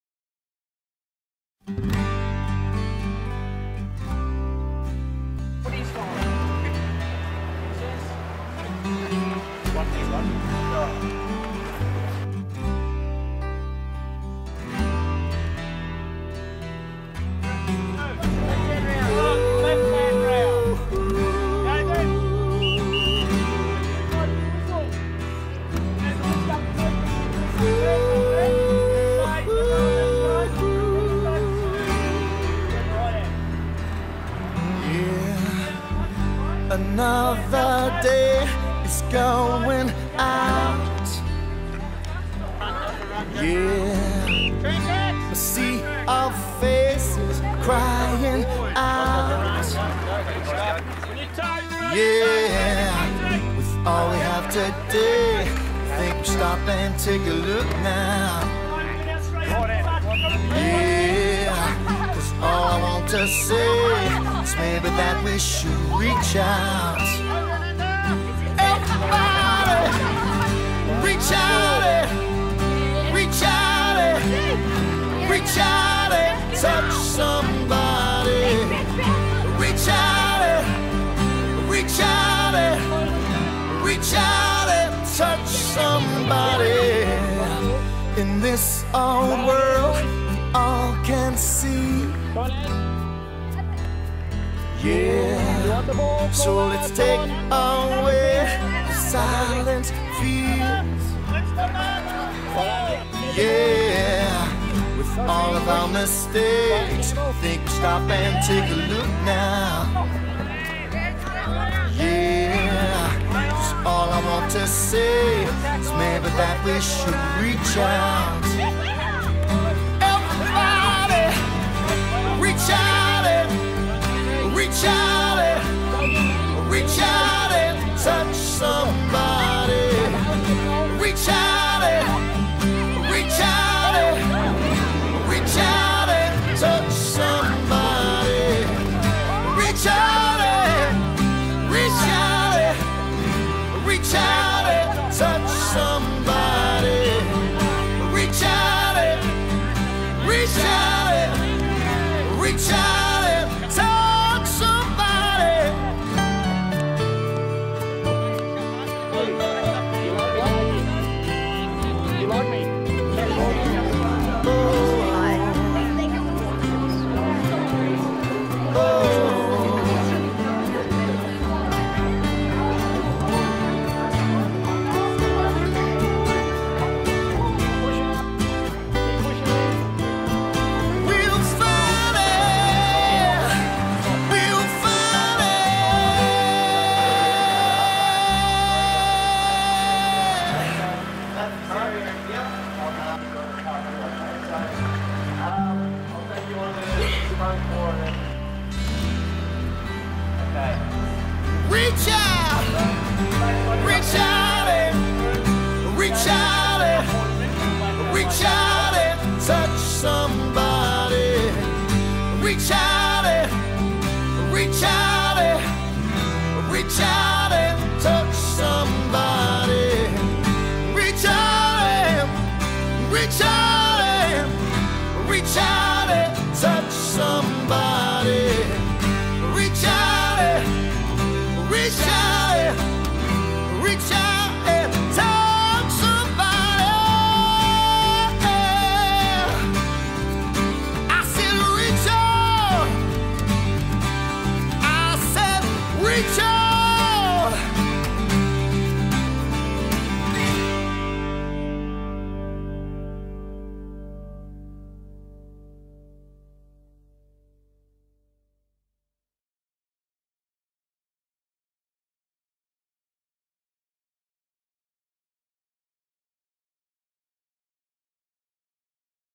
DFS visits Collingwood FC
The DFS STP program visited the Westpac Centre, home of the Collingwood Football Club, for an exclusive tour and training session.